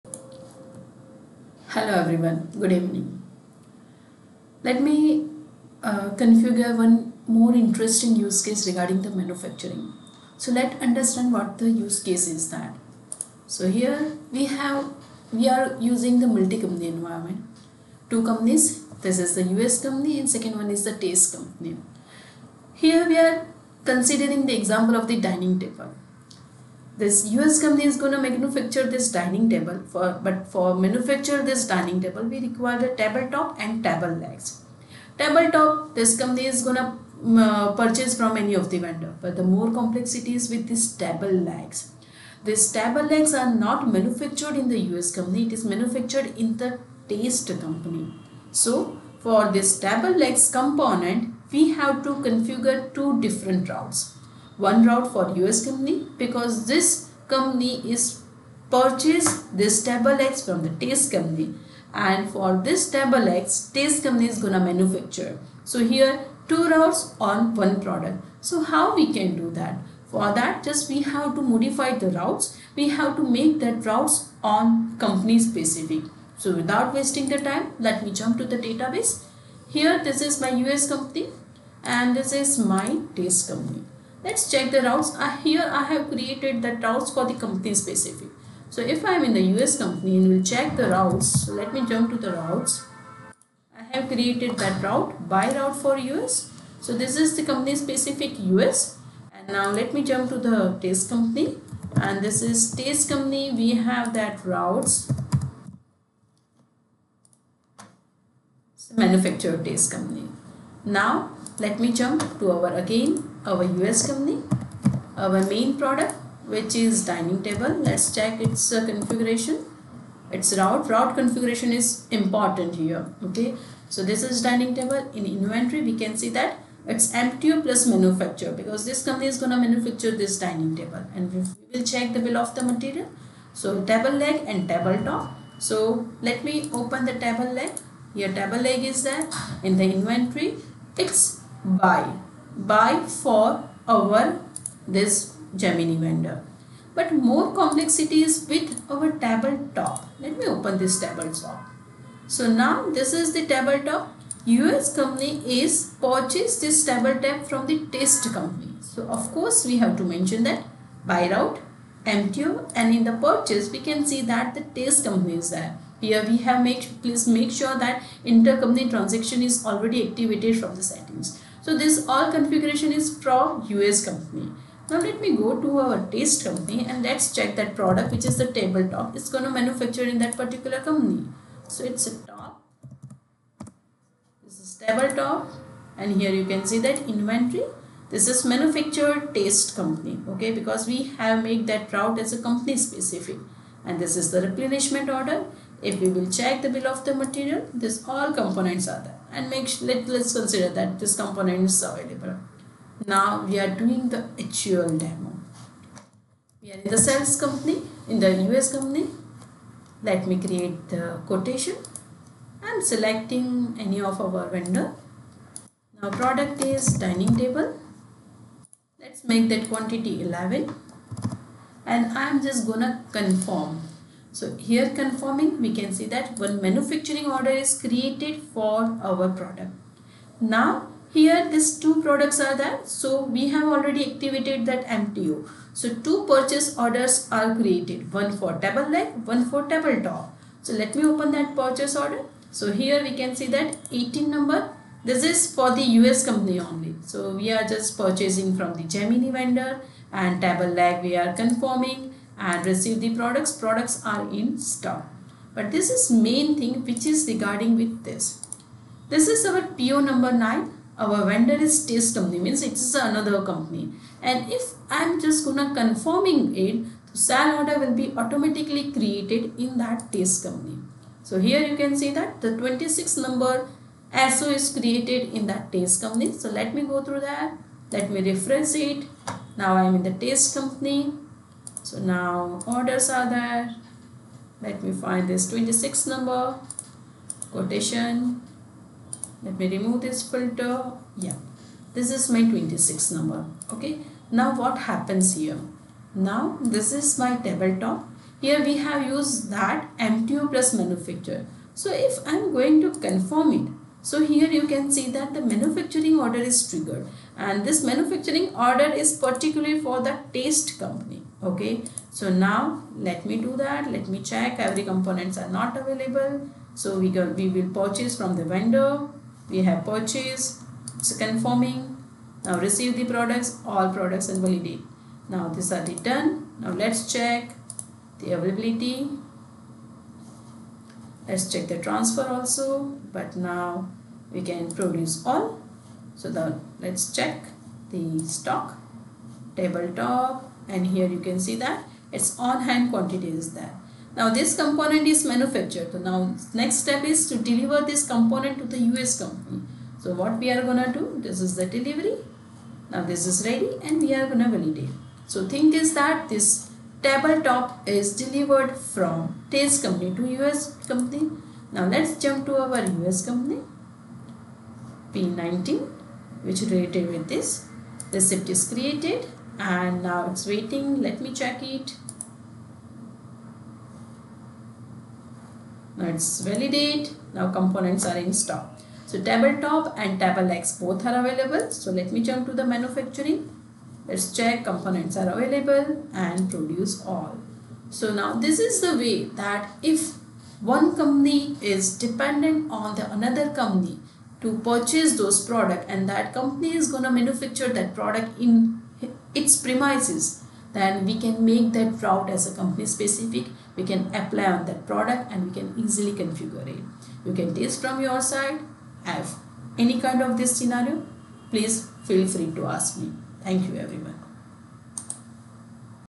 Simplifying Manufacturing Orders Management in multi-company setup by modifying the Routes
When managing manufacturing in a multi-company environment within @Odoo  you can set up company-specific routes and workflows to efficiently handle scenarios where one company manufactures the finished product, and another produces a key component. Here's a simplified guide:

Scenario:
US Company: Responsible for manufacturing the main finished product, a Dining Table.
Test Company: Produces a semi-finished component, the Tabletop, which is part of the Bill of Materials (BOM) for the Dining Table.

Process Workflow
The US Company starts manufacturing the Dining Table.
If a Tabletop is needed, a purchase order is created for the Test Company.
The Test Company manufactures the Tabletop and ships it to the US Company.

-Simplifies manufacturing and procurement across companies.
-Automates communication between the two companies.
-Ensures a clear workflow for managing semi-finished and finished products.

odoo